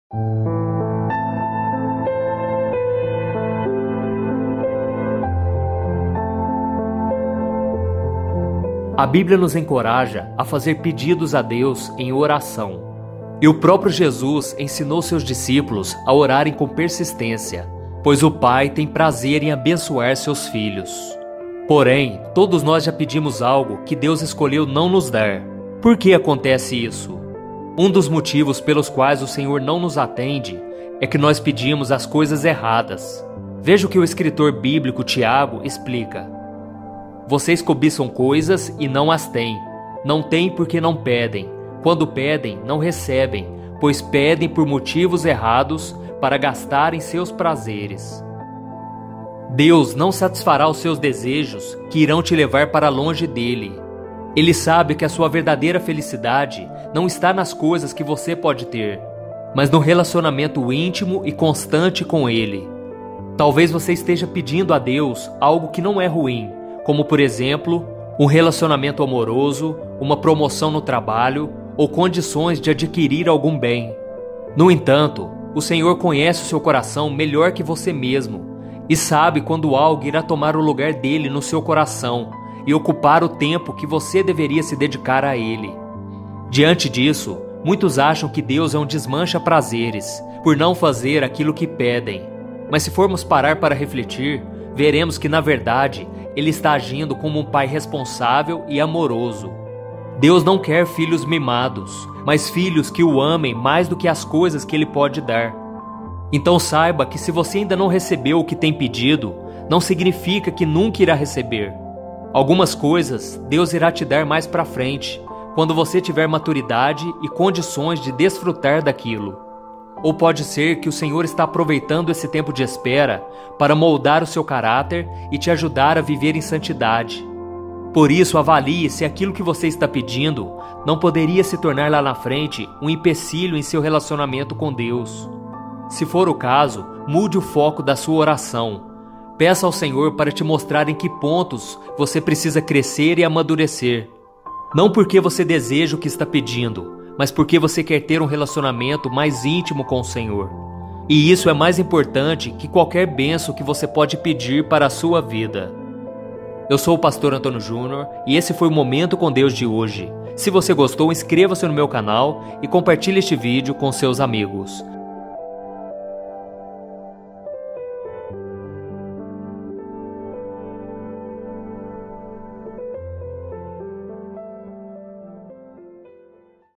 SAIBA PORQUE VOCÊ NÃO TEM RESPOSTA DE ORAÇÃO
Se você não está tendo resposta de oração, assista este vídeo e saiba porque muitas vezes Deus não responde nossa oração, mesmo quando fazemos tudo certo.

A Bíblia nos encoraja a fazer pedidos a Deus em oração para termos respostas. E o próprio Jesus ensinou seus discípulos a orar com persistência, pois o Pai tem prazer em abençoar seus filhos (Mateus 7:11). Mas todos nós já pedimos algo que Deus não nos deu. Saiba porquê!

📖 Conheça meu Curso online!

2021